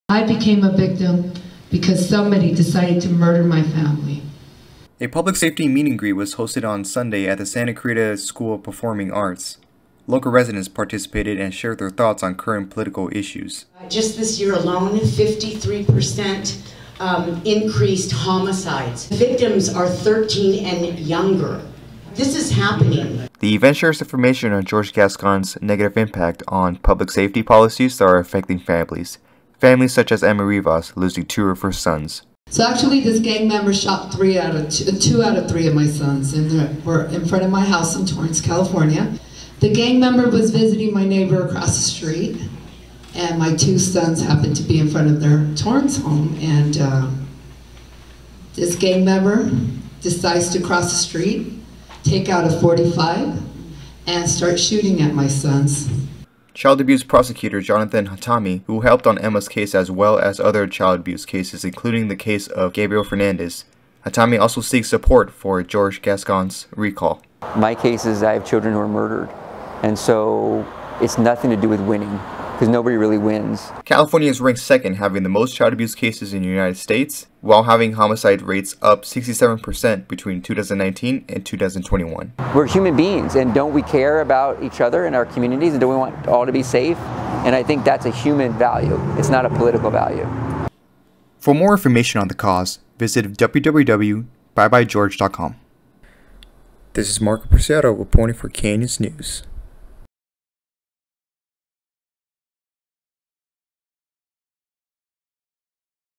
PUBLIC SAFETY
An event supporting the recall of District Attorney George Gas-Cone brought together crime victims and a prosecutor who's leading the charge to remove his boss from office. Marco Preciado reports in from the townhall discussion.